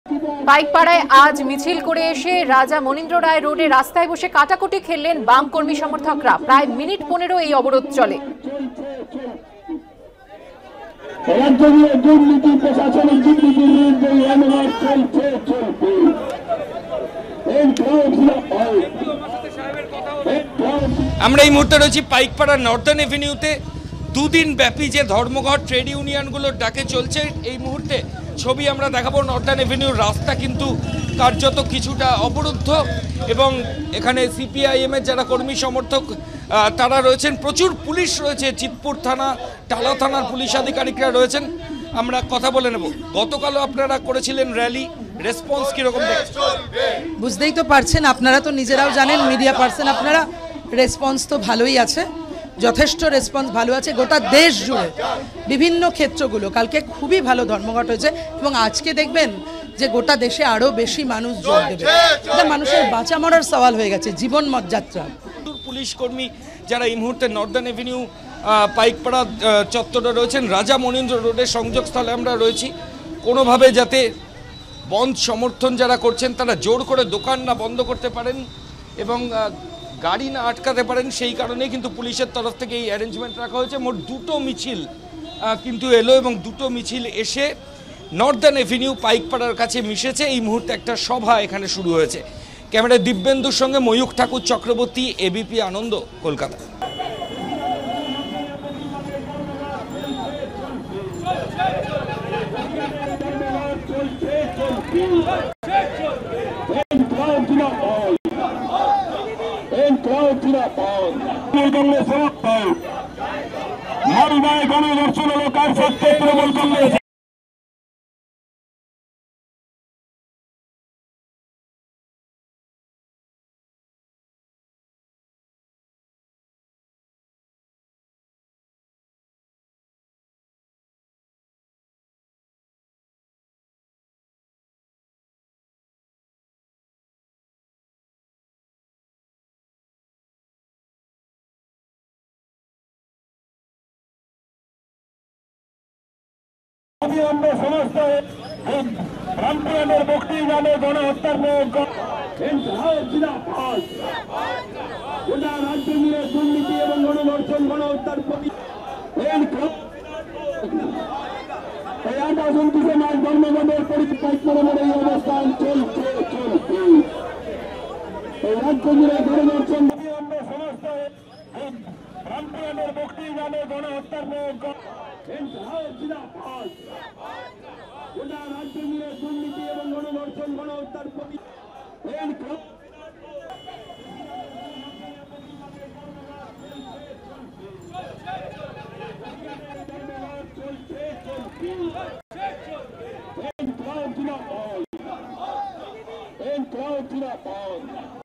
Bharat Bandh: পাইকপাড়ায় রাস্তায় বসে কাটাকুটি খেললেন বাম কর্মী-সমর্থকরা।Bangla News
পাইকপাড়ায় আজ মিছিল করে এসে রাজা মণীন্দ্র রায় রোডে রাস্তায় বসে কাটাকুটি খেললেন বাম কর্মী-সমর্থকরা।  প্রায় মিনিট ১৫ এই অবরোধ চলে।